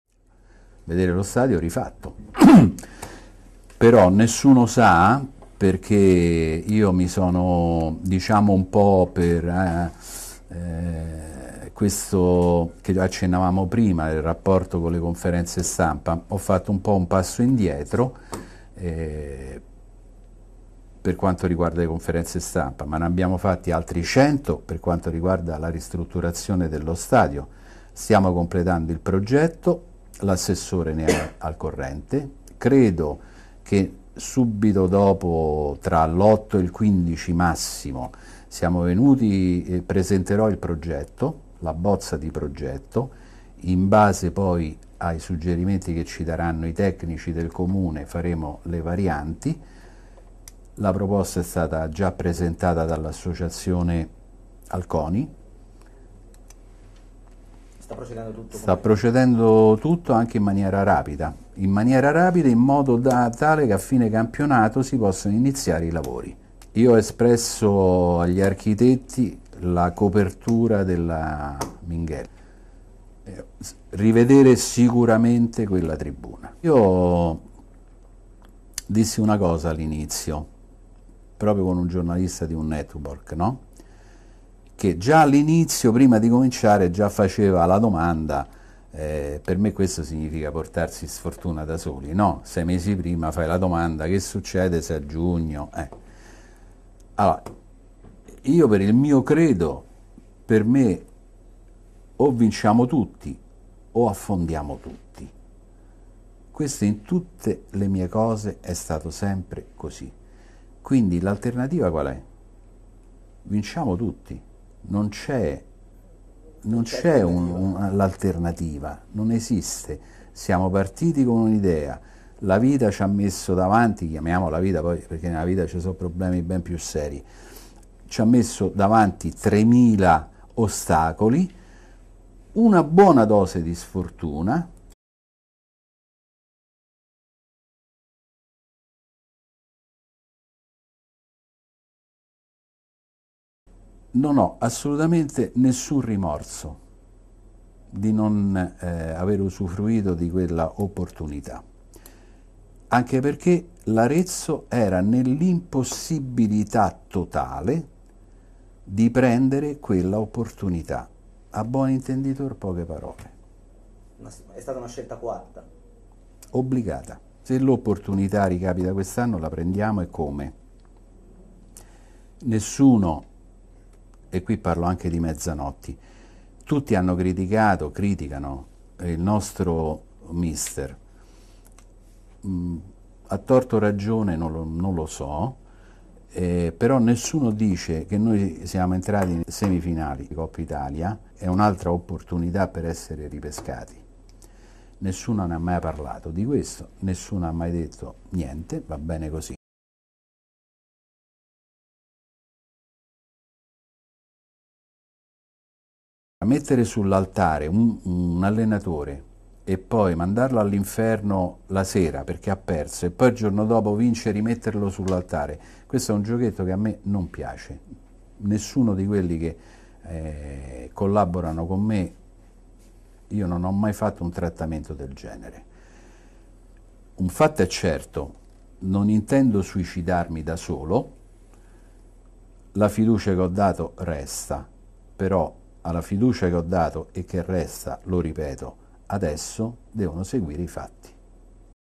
Il presidente dell'Arezzo Ferretti in un'intervista a tutto tondo
"Un anno entusiasmante e duro perchè non è stata una passeggiata" ha detto il presidente Ferretti nell'intervista realizzata dall'ufficio stampa dell'US Arezzo in occasione del primo anno di presidenza dell'imprenditore romano che il 9 gennaio 2013 si presentò nella sala stampa del Comunale dopo aver rilevato parte delle quote in mano alla Sport&Events. "Il 9 gennaio è il compleanno di mia moglie e non so se le ho fatto un regalo gradito -- prosegue il presidente. Fu l'allora proprietario dell'Arezzo a prendere l'appuntamento, non fu certo idea mia. Mia moglie mi sprona sempre a fare meglio, è una buona vitamina per questo".

Tra gli argomenti toccati negli oltre 40′ di intervista il tifo, lo stadio (un palcoscenico di tutto rispetto per la Serie D) e il futuro dell'Arezzo qualora la Pistoiese dovesse mettere a segno il colpo del ko domenica prossima.

tifo
I tifosi troppo pessimisti. "Sono molto esigenti e vorrebbero che tutto funzionasse al cento per cento. Purtroppo non è sempre così perchè nella vita non tutto va come noi vorremmo. Ho una critica da fare agli aretini: credo che siano troppo pessimisti. Scrivono che non vogliono gufare però scrivono pensieri negativi. Se non ci crediamo la battaglia è persa in partenza. Ho avuto un incontro con la tifoseria, quella vera, e con molto rispetto mi hanno detto cosa pensano. Loro sanno qual è la mia idea. Credo in questo progetto e lo confermo di nuovo, ma ribadisco che non sono dedito al suicidio e oggi pretendo risposte ancora più certe".

Il futuro, cosa accadrà all'Arezzo dopo maggio. "Io all'inizio dissi una cosa: o vinciamo tutti o affondiamo tutti. L'alternativa è vinciamo tutti. Siamo partiti con un'idea poi davanti abbiamo trovato 3mila ostacoli e una buona dose di sfortuna. Al tifoso che mi chiede cosa accadrà gli dico di credere nel progetto Arezzo, prima di giugno c'è molto tempo.

La domanda di ripescaggio non presentata. "Non ho alcun rimorso di non aver usufruito di quella possibilità anche perchè l'Arezzo non avrebbe potuto far fronte al ripescaggio. Se quest'anno ci sarà l'occasione non ce la faremo sfuggire".

Partite sofferte, giocatori, staff tecnico. "Soffro sempre quando sono in tribuna e la cosa che mi fa soffrire ancora di più sono i nostri secondi tempi quando poi riusciamo a recuperare. La partita più bella che ho visto è stata quella con il Siena che è nata per gioco e poi abbiamo vinto. Un giocatore che preferisco? Amo tutti i miei giocatori anche se alcuni hanno reso sotto le aspettative. Ho conosciuto Enrico De Martino per motivi di lavoro diversi anni fa. E' uno di quelli con cui puoi parlare dei problemi senza arrivare allo scontro a differenza di molti suoi colleghi. Mezzanotti? Non intendo suicidarmi da solo, ho dato fiducia e questa resta ma adesso devono seguire i fatti".

Ferretti, Fanfani, Romizi
Ferretti, Fanfani, Romizi

ARTICOLI CORRELATI
L'AREZZO VERSO PISTOIA: MEZZANOTTI RISCOPRE BRICCA TERZINO E LANCIA NELLA MISCHIA RASCAROLI
I rapporto con le istituzioni. "Le autorità mi hanno accolto molto bene. Il primo incontro fu con il sindaco ma non parlamo di cosa avrei voluto fare nel tempo. Il rapporto più costruttivo è nato con l'assessore Romizi a cui piace lo sport, è giovane, semplice".

Il progetto per lo stadio: la copertura della "Curva Minghelli". "Vogliamo intervenire sullo stadio. Abbiamo già presentato il progetto al CONI e al Comune. Aspettiamo il parere dei tecnici. Una sorpresa per i tifosi? Dotare di una copertura la Curva Sud, è nel progetto".

Zerbini, Ferretti, Locatelli
Zerbini, Ferretti, Locatelli

Il settore giovanile. "Da come me ne parla il direttore generale è importante. Dobbiamo solidificare il rapporto con il Villaggio Amaranto in modo da arrivare velocemente alla matricola unica".

I rapporti con Orgoglio Amaranto. "Può vedere sempre la documentazione. Mi prendo la colpa di aver preso decisioni senza comunicarle al comitato. Purtroppo gli impegni quotidiani sono sempre tanti ed è difficile stare dietro a tutti. Comunque ci siamo chiariti. A mio avviso l'azionariato popolare è il futuro".